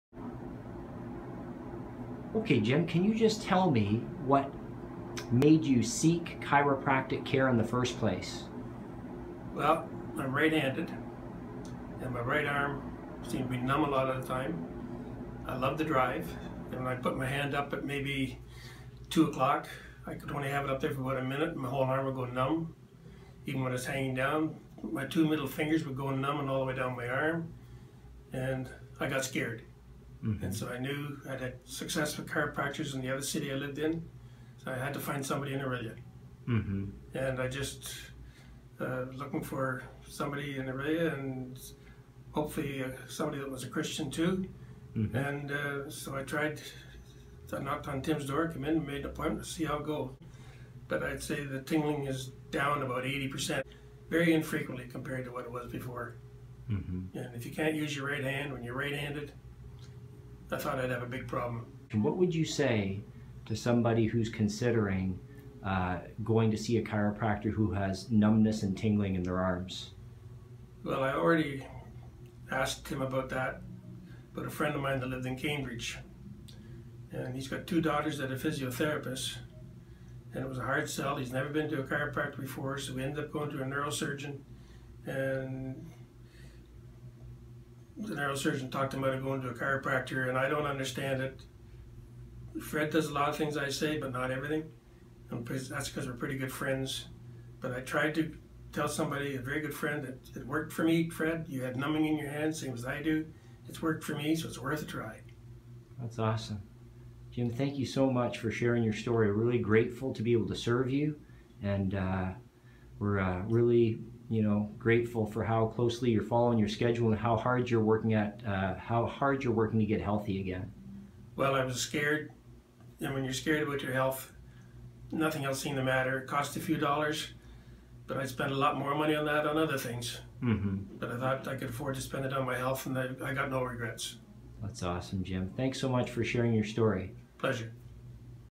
Grace Chiropractic Orillia Reviews - Patient Testimonial
A satisfied patient has sensation and strength down his right arm restored through chiropractic care